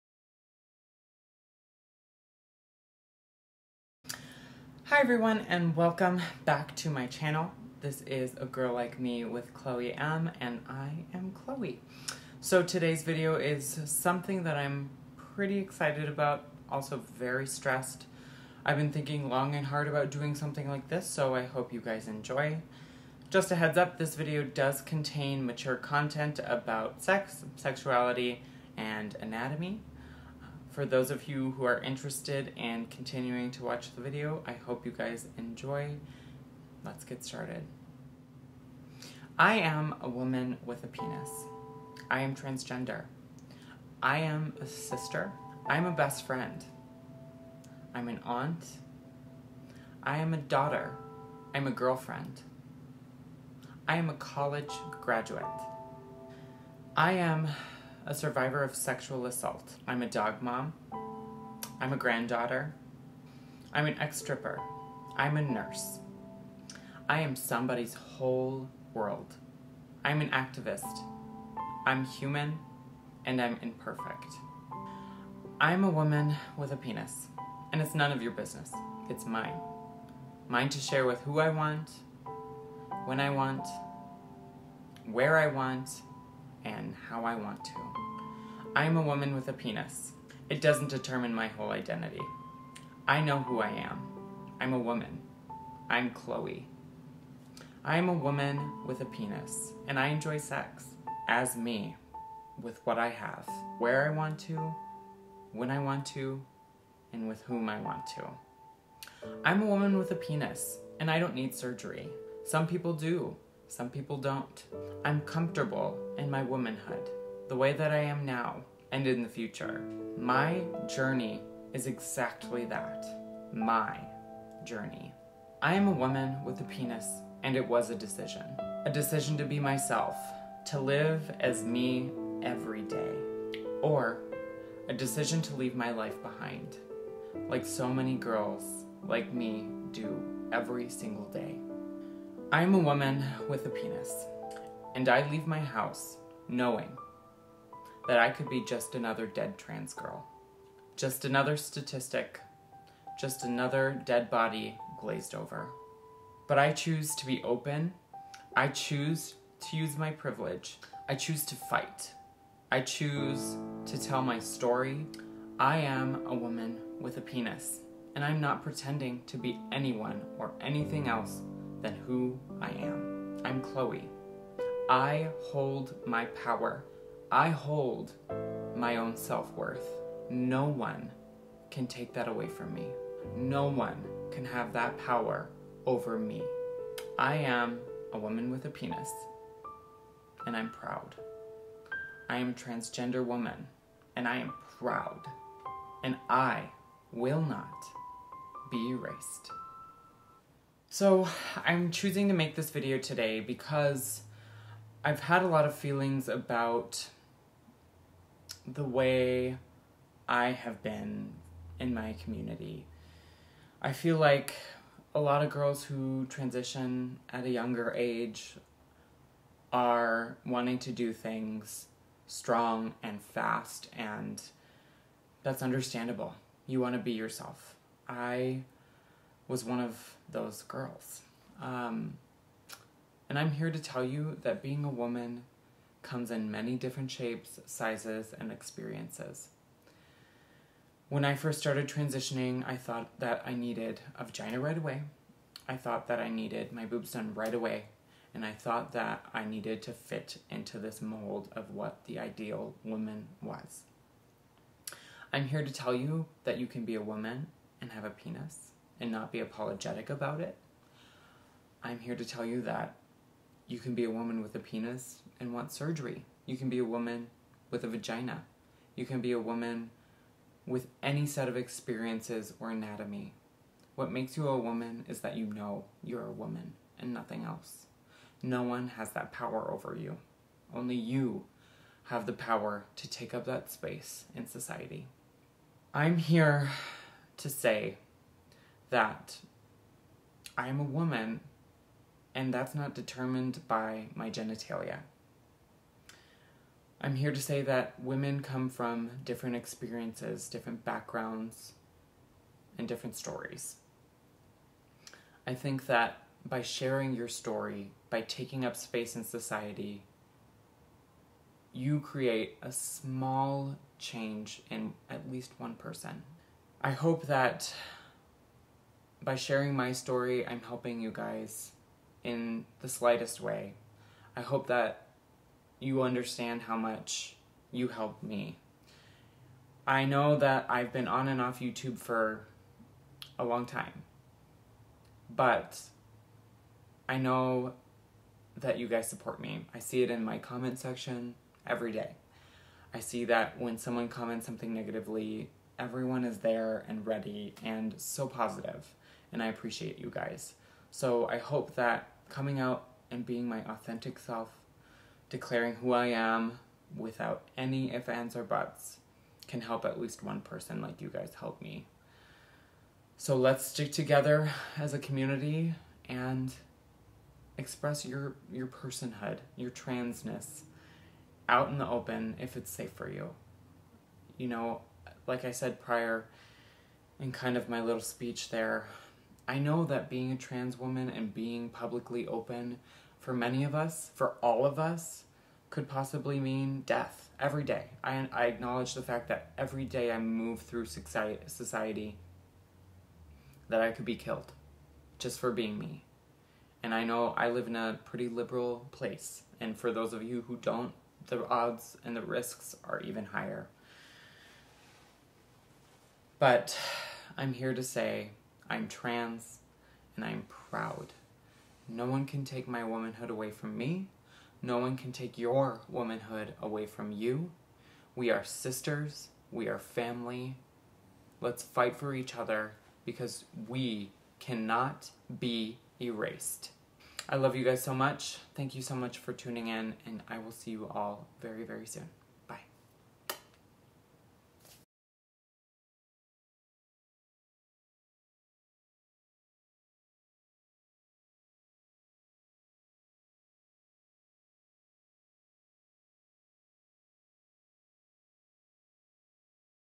I Am A Woman With A Penis
Hey everyone,

I wanted to touch on something that I have been thinking about a lot lately. My womanhood is not based on my genitalia. My womanhood is determined by me, and no one else. It is that simple. For all trans woman who are told daily by society that you are not a "real woman" just remember they don't hold your power, you do.  I hope you all enjoy. Thanks for all your positivity!

XOXO,
Chloe M.